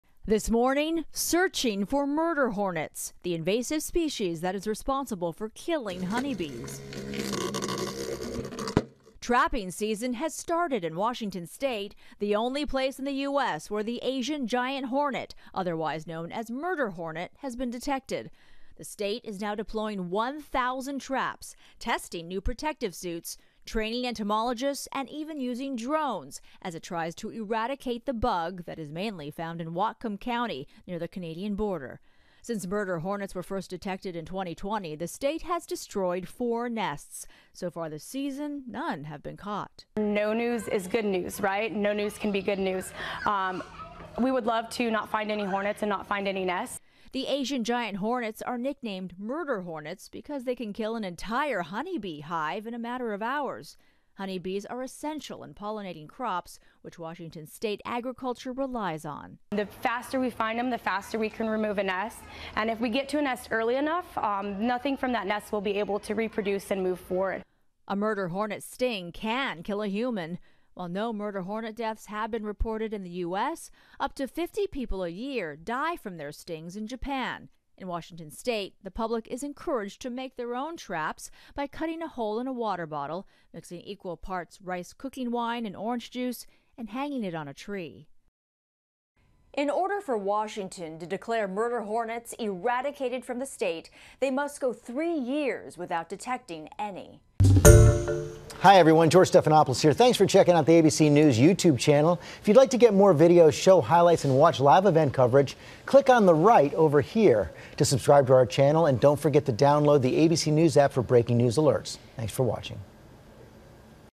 Hunting murder hornets
The search for thousands of these very efficient killers is underway in Washington state. ABC News’ Andrea Fujii reports.